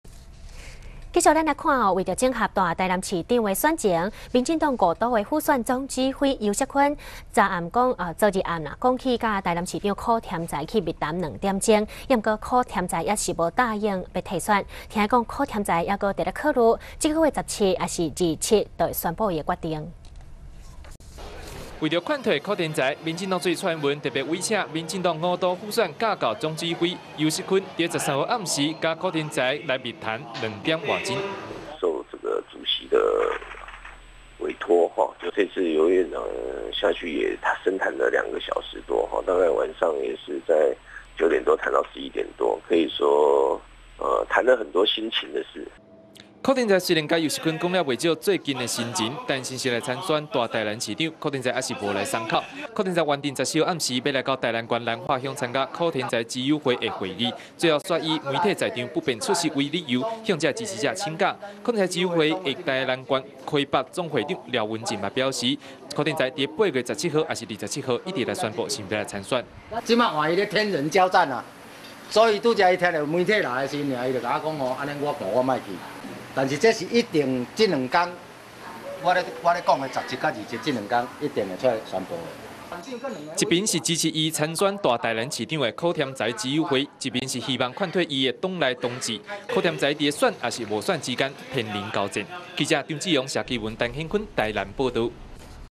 2010-08-15公視中晝新聞(游錫(方方土)闢室密談 勸退許添財參選)
為了勸退許添財，民進黨主席蔡英文特別委請民進黨五都輔選架構總指揮游錫(方方土)，在13號晚間南下，和許添財闢室密談兩個多小時。

許添財雖然和游錫(方方土)談了不少最近的心情感受，但是否會參選大台南市長，許添財還是沒有鬆口。而許添財原訂14號晚上要到台南縣南化鄉參加許添財之友的會議，最後卻以媒體在場，不便出席為由，向這些支持者請假。許添財之友會的台南縣溪北總會長廖文振也表示，許添財在8月17號或27號，一定會宣布是否參選。

一邊是支持他參選大台南市長的許添財之友會，一邊是希望勸退他的黨內同志，許添財在選與不選之間，天人交戰。
記者 張志雄 謝其文 陳顯坤 台南報導"